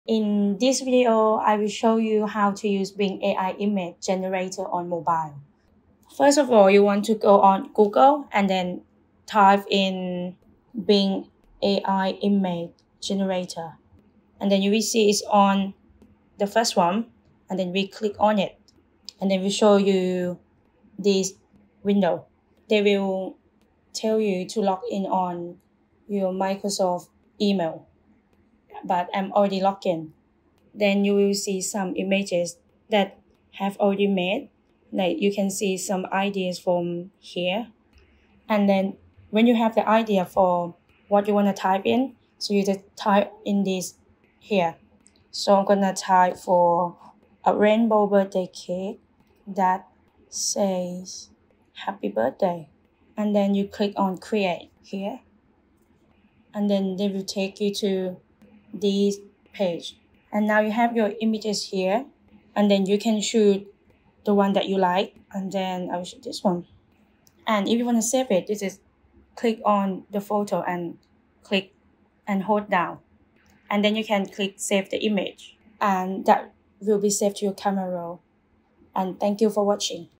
How To Use Bing AI Image Generator On Mobile
In this video, I show you how to use Bing AI Image Generator On Mobile. It is a free tool that's perfect for creating good AI images.